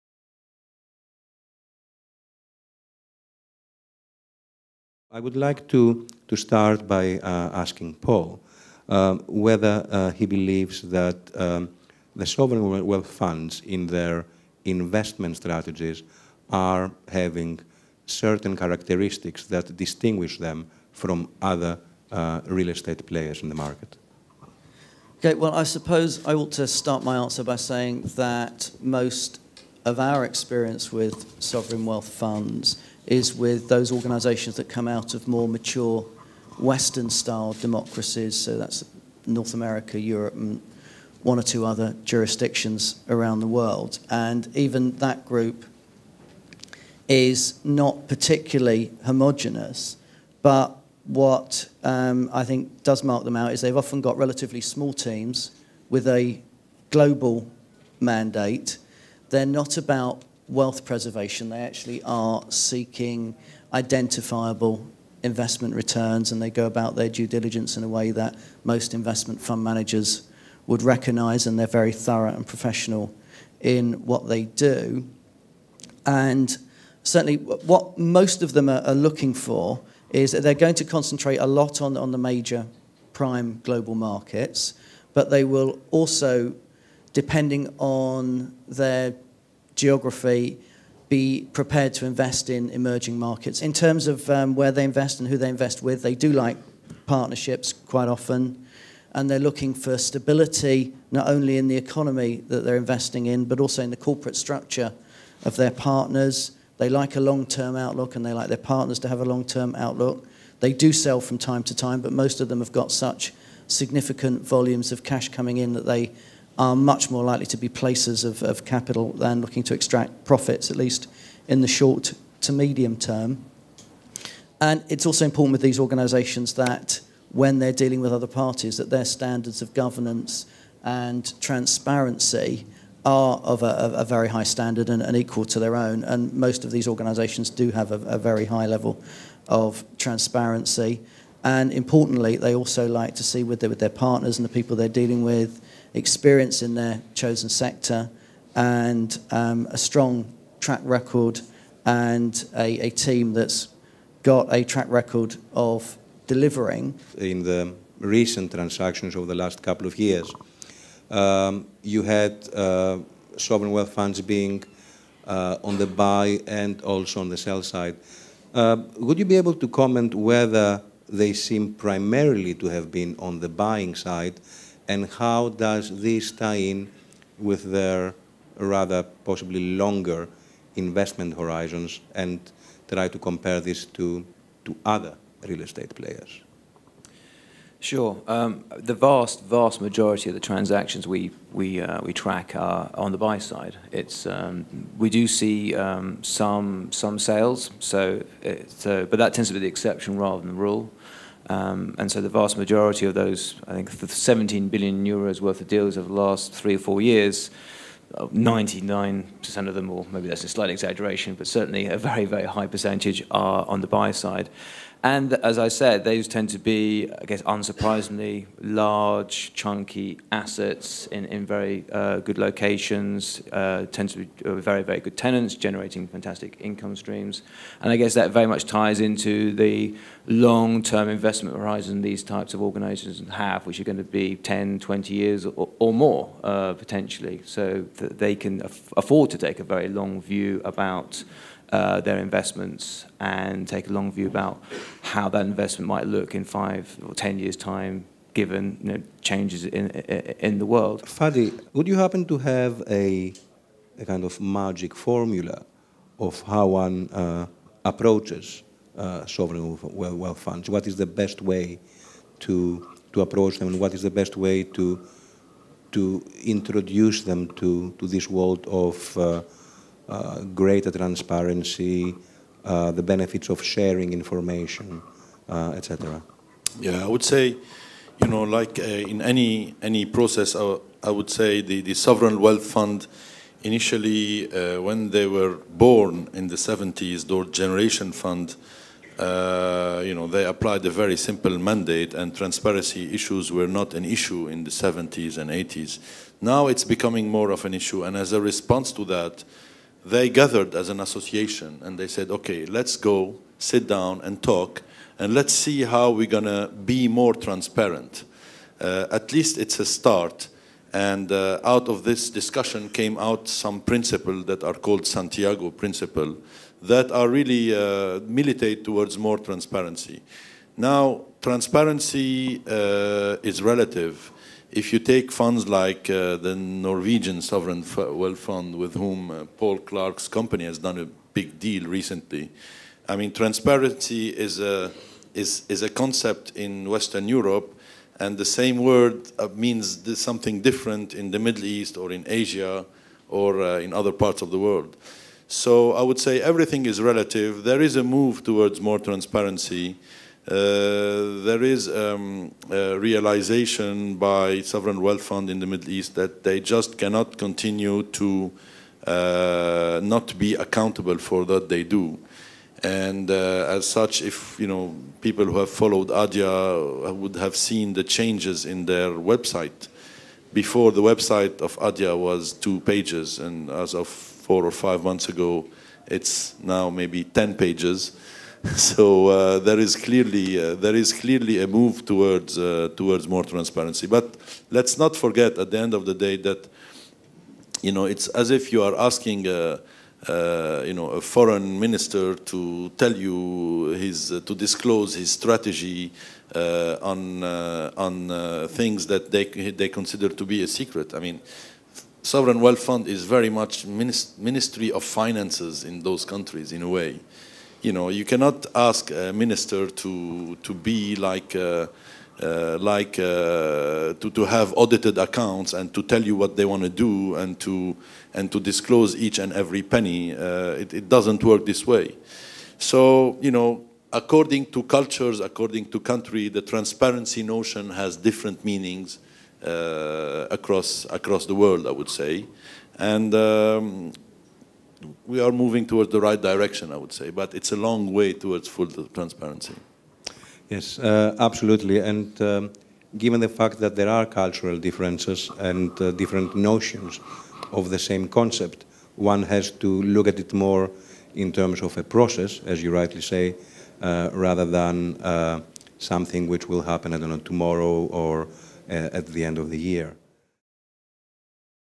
MIPIM 2011 - Capital requirements: understanding what sovereign wealth funds are looking for - EDIT
Sovereign wealth investors are becoming an increasingly important source of capital for European real estate. These investors clearly like property, but what sort of property? This session brings together key market players who have worked extensively with sovereign wealth investors, to make clear where -- and with whom -- their capital will be placed from now on.

Moderator
Nassos Manginas, Head of Business Development, Continental Europe, IPD (UK)

Speakers
Fadi Moussalli, Regional Director -- International Capital Group, Jones Lang LaSalle (UAE)
Richard Yorke, Managing Director EMEA, RCA (UK)
Paul Clark, Director of Investment & Asset Management, The Crown Estate (UK)